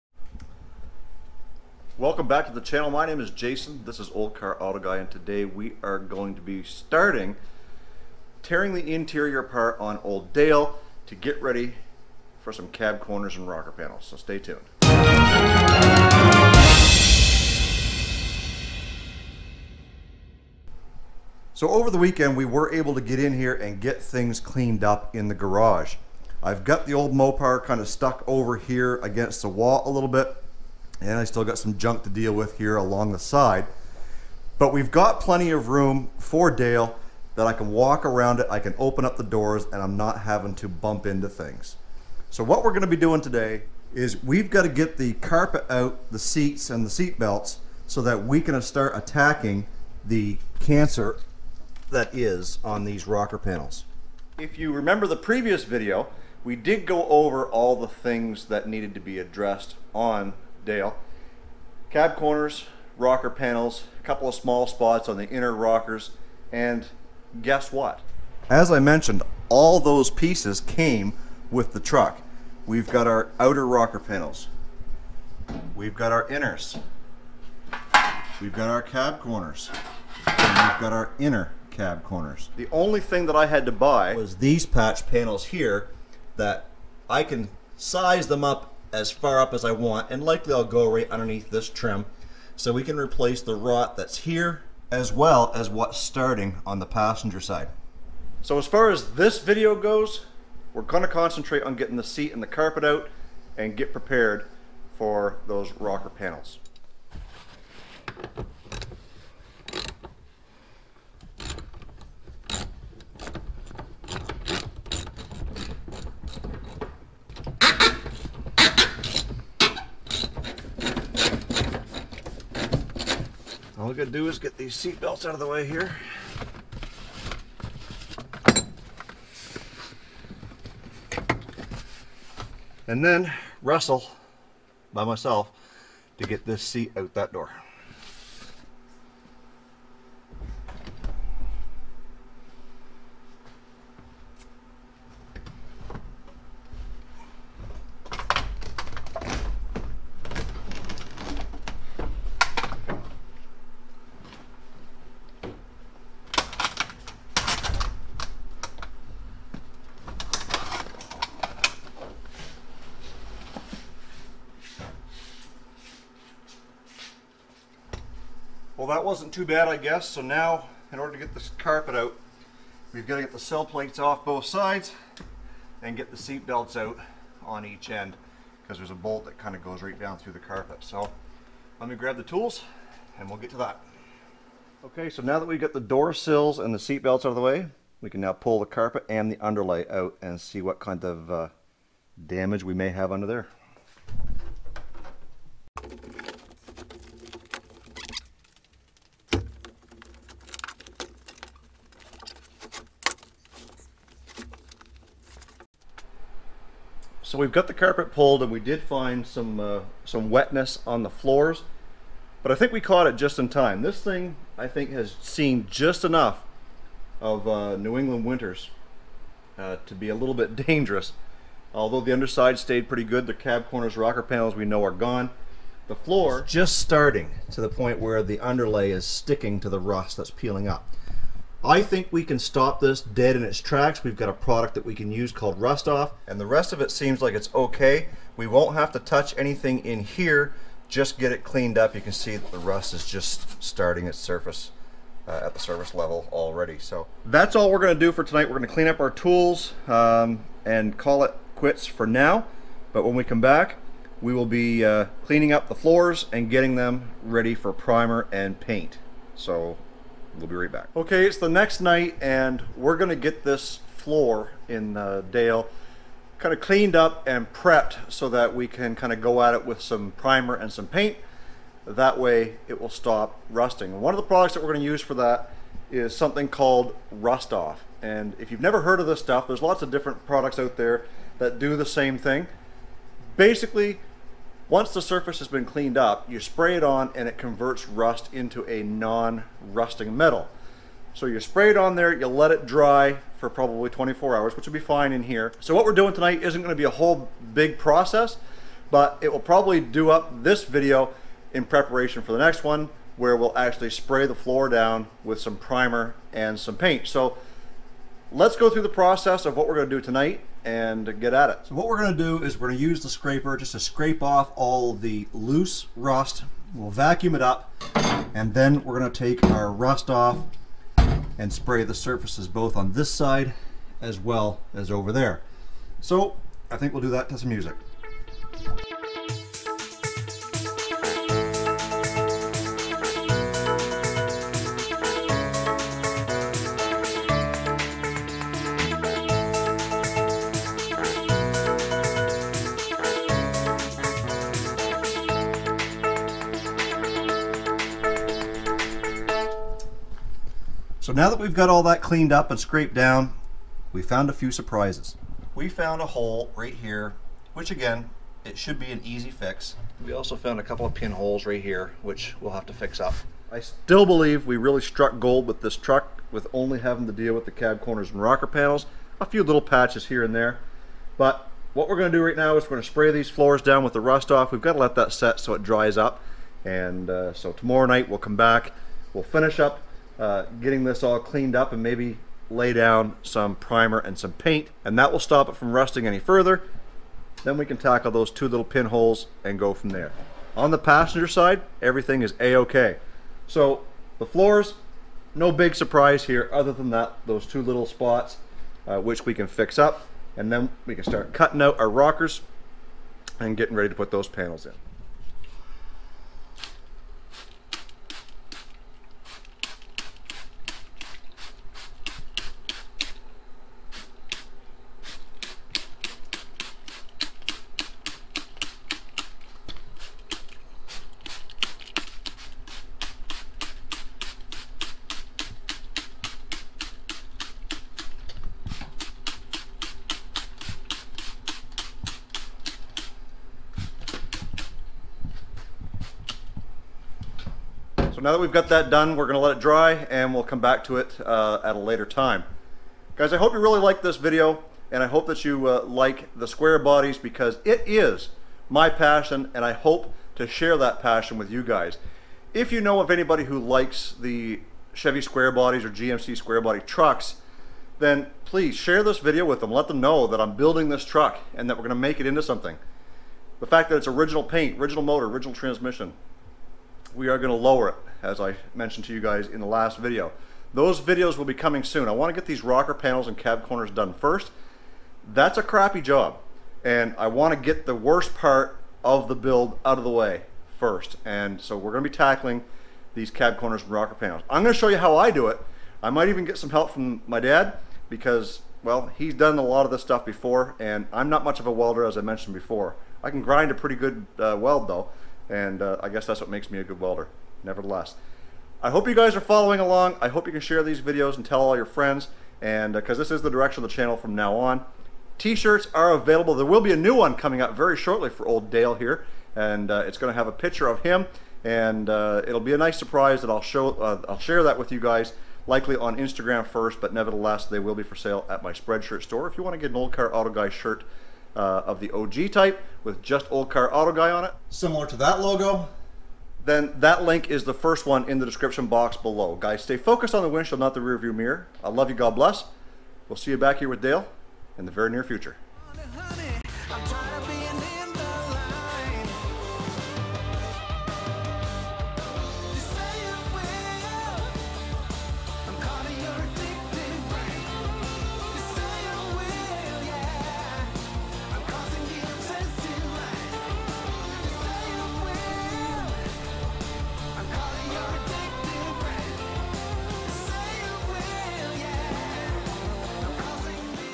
HOW TO REMOVE THE SEATS, CARPET AND SEAT BELTS IN A 1973 1987 CHEVROLET C10 PICKUP
In this installment, I'll show you how to remove the seats, carpet and seat belts in my 1977 Chevy C10 square body pickup as we dive into preparation for new cab corners and rocker panels. I found the floor to be a bit rusty due to wet carpets so we'll clean them up and treat them with Rust-off. Then I'll be ready for primer and paint.

THIRD LINK
TUBEBUDDY

Jason @oldecarrautoguy
220 Route 170

or